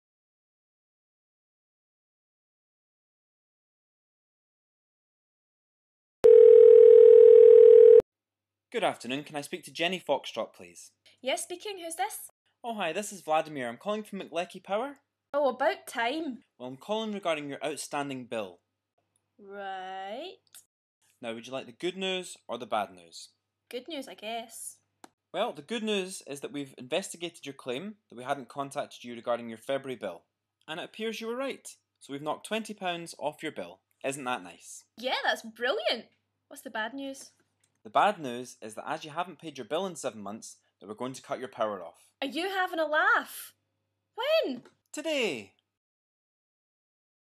The Mc Lecky Incident
The Mc Lecky power company call a customer to tell them that they have some good news and some bad news regarding their outstanding bill.